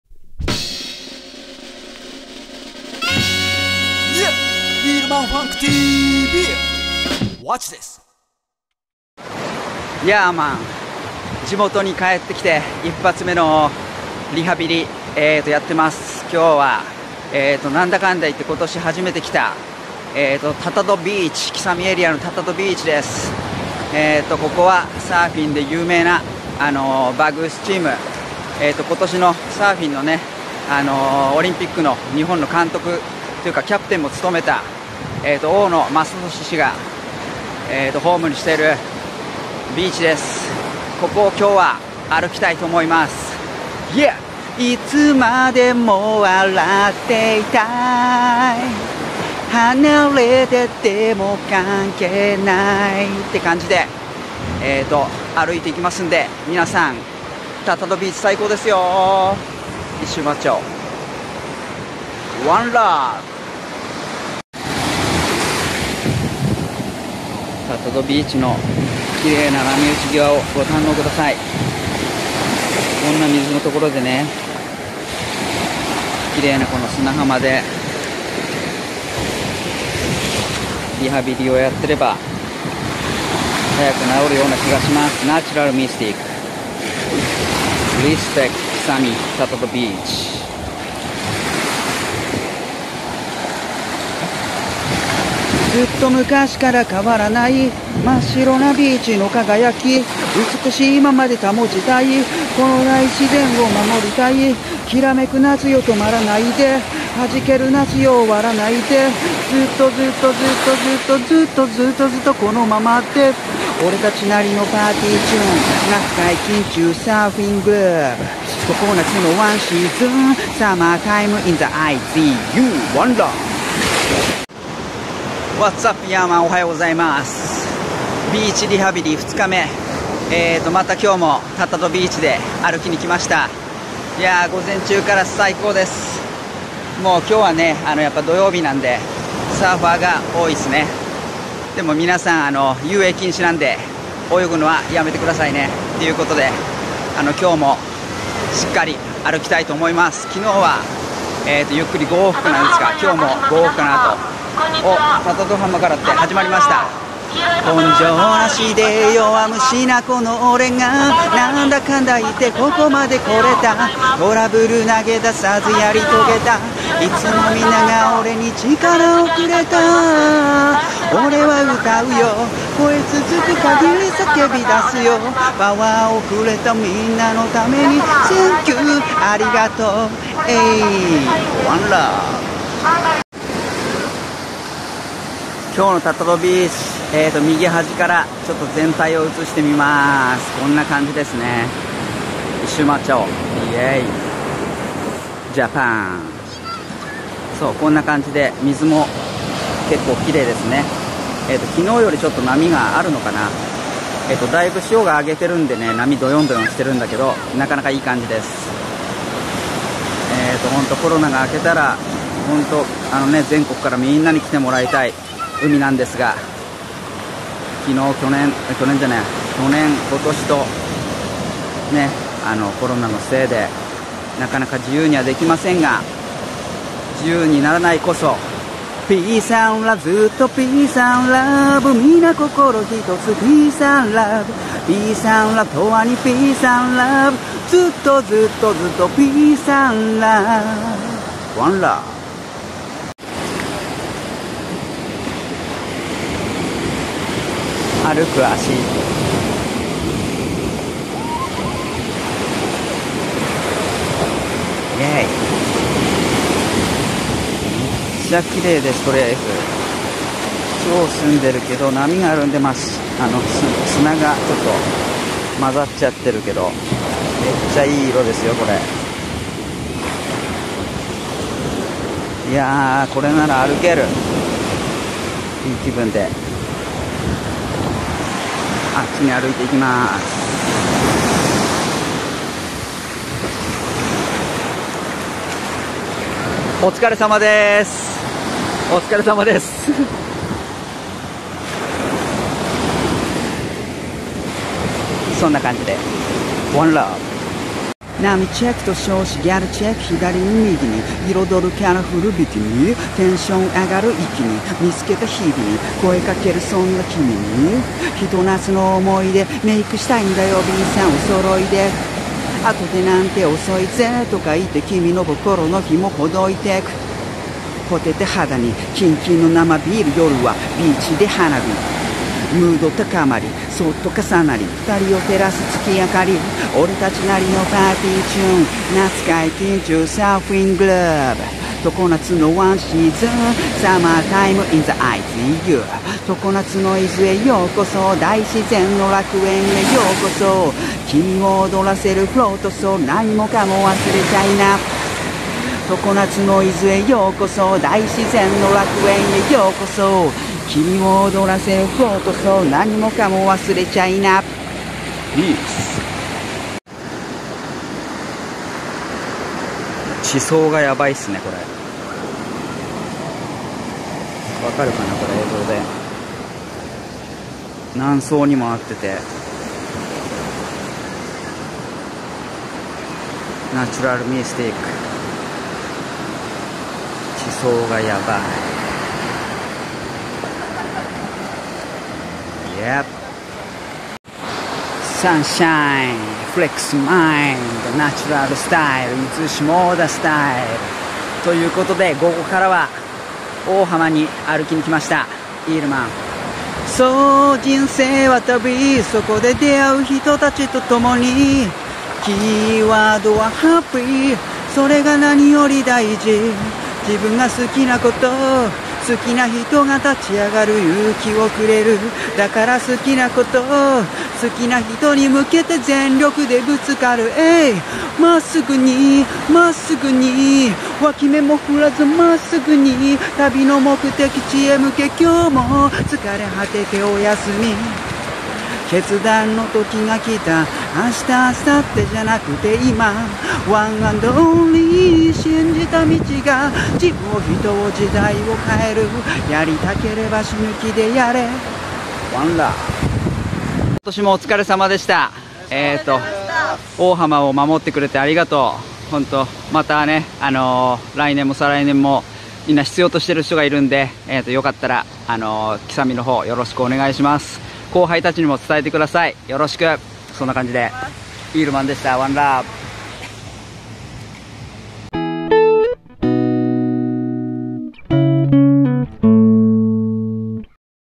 リハビリbeach walking👣 リハビリネイチャーガイド🏝️start🤙１日目～3日目 下田市吉佐美tatado beach～ohama beach🏝️
どうも～EELMANです🎵
YouTube久々です😅
8月中旬、手術の為に入院後、リハビリを楽しくやるためにまずは地元の自慢の綺麗なbeachを歩きながら、A Cappellaで歌いながらネイチャーガイドもやってしまおうとノリでインスタで始めたリハビリウォーキングシリーズ👣

伊豆半島の魅力✨よかったらお楽しみください🎵

ONE LOVE🤗